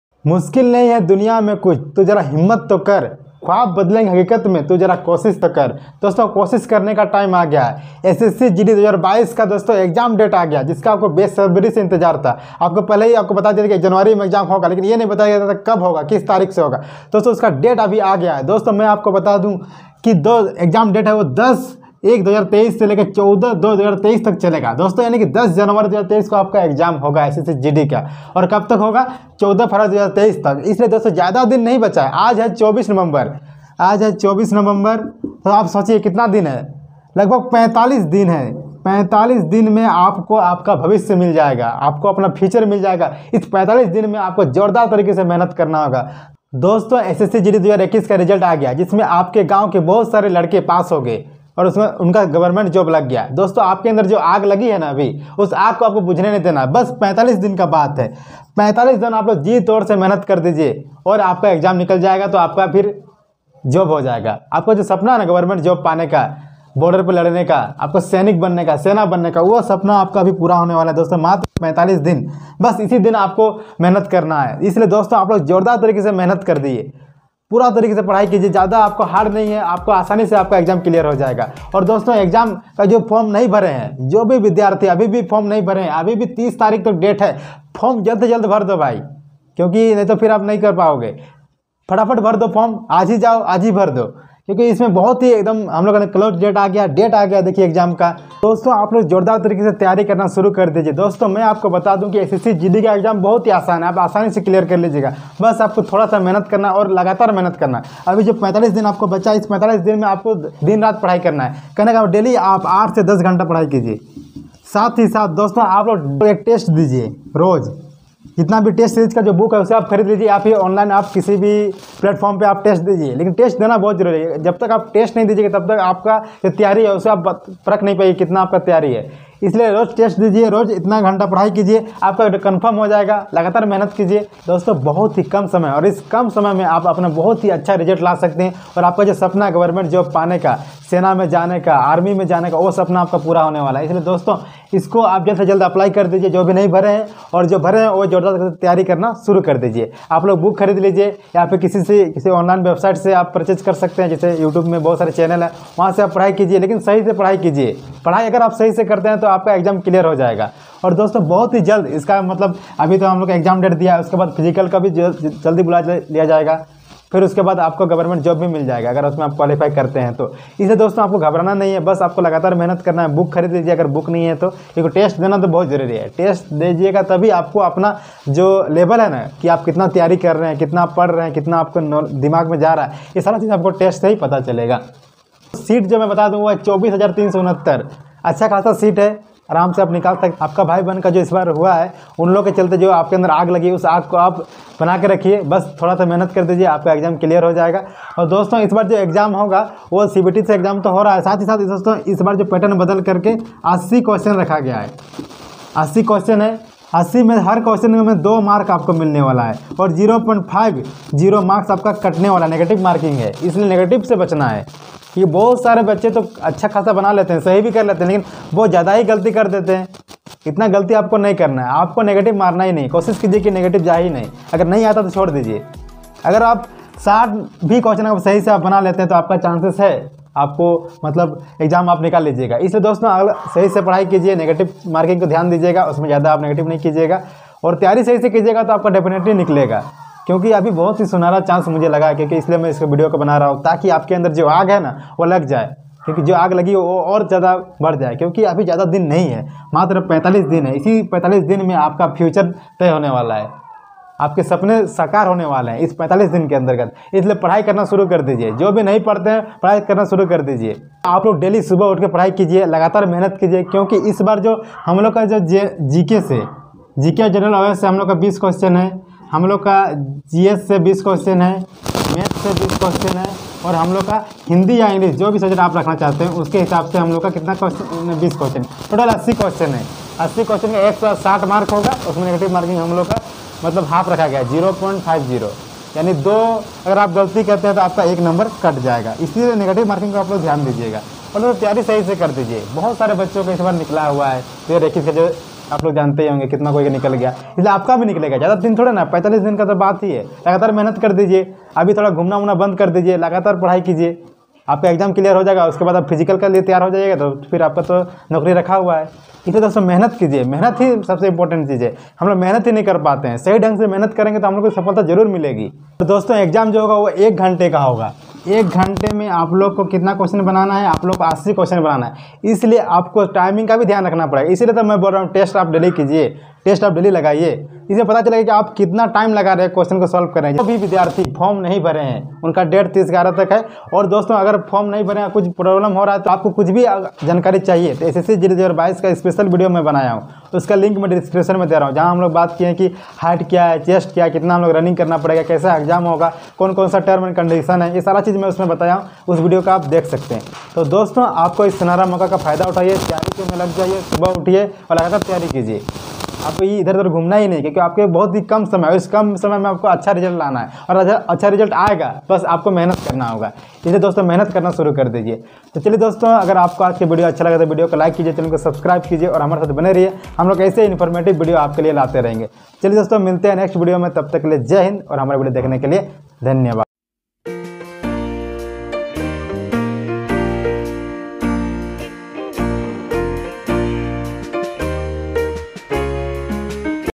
SSC GD 2022 EXAM DATE
इस वीडियो में हमलोग "SSC GD 2022 EXAM DATE" के बारे वर्णन किए हैं।
इस चैनल में विद्यार्थियों को सही मार्गदर्शन, विशेष स्टोरी आगे बढ़ने के तरीके के बारे जानकारी प्राप्त करते हैं,